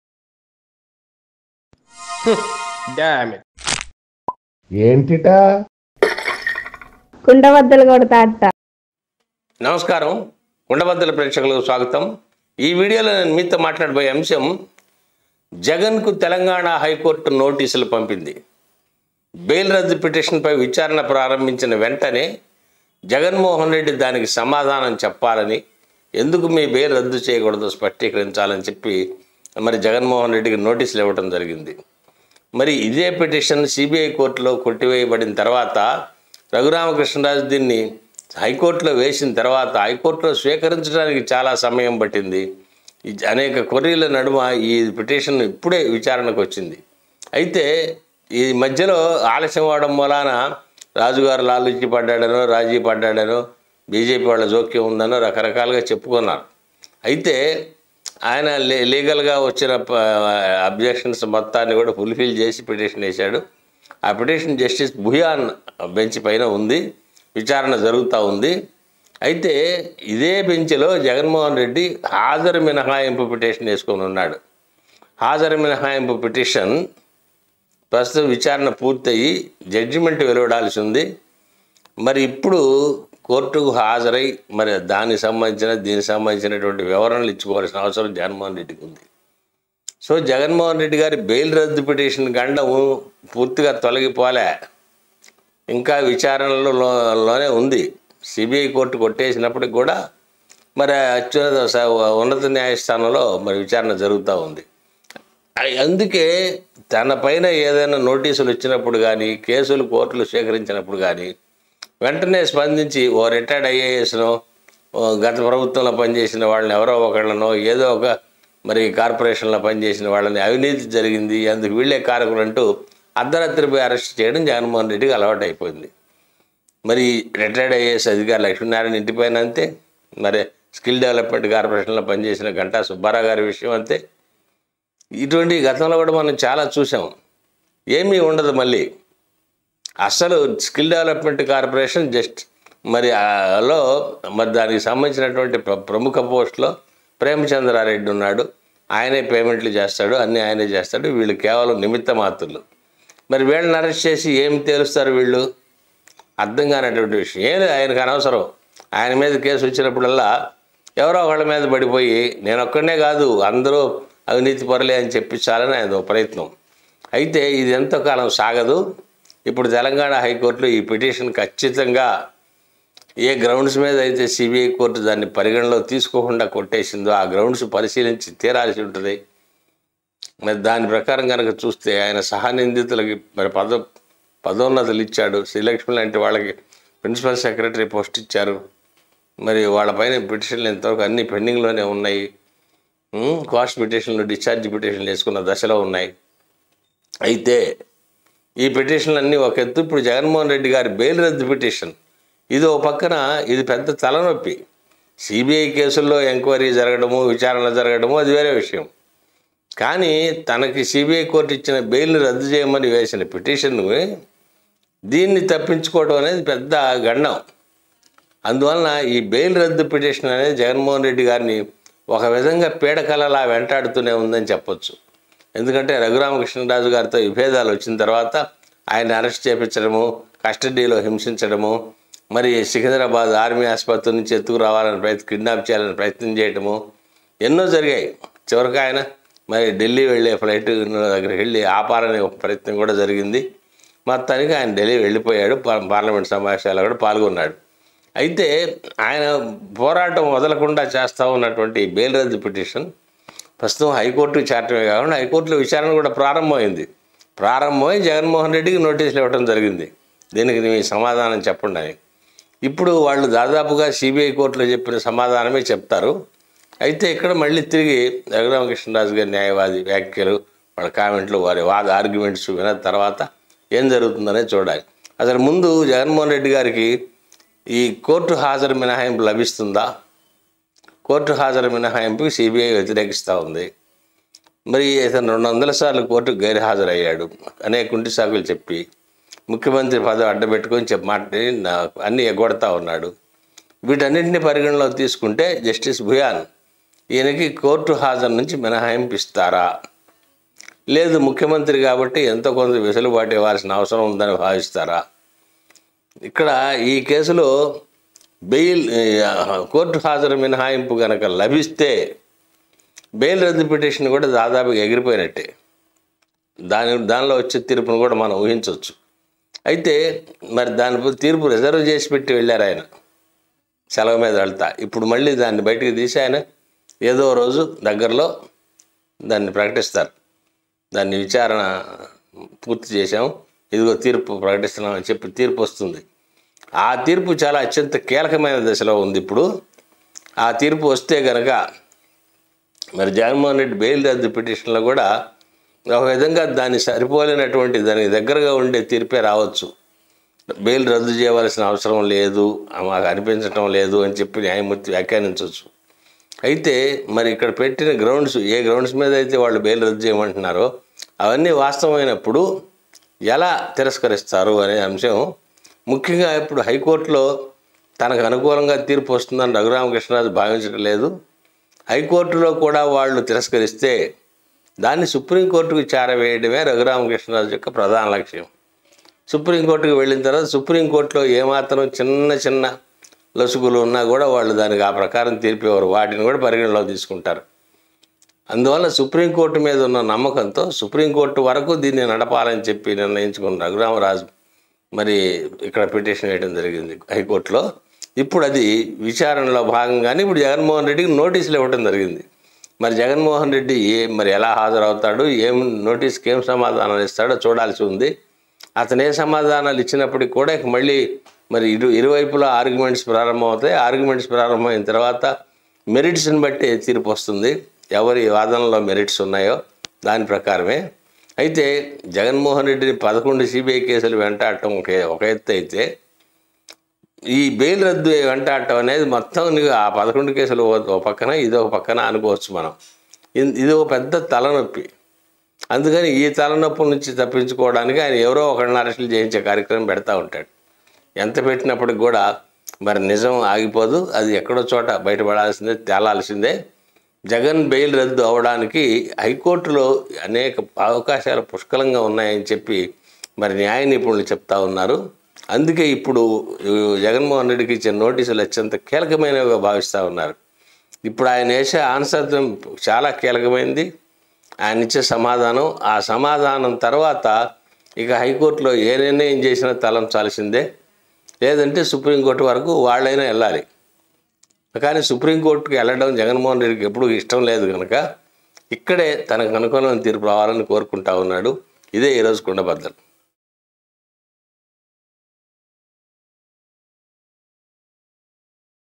జగన్ కు తెలంగాణ హైకోర్టు నోటీసులు | జగన్ బెయిల్ రద్దు ఖాయమేనా? | YS Jagan | Telanga High Court | CBI
Raghurama Krishna Raju,
Kundabaddalu,
AP Highcourt,
Telangana Highcourt,
CBI,
AP Three Capitals,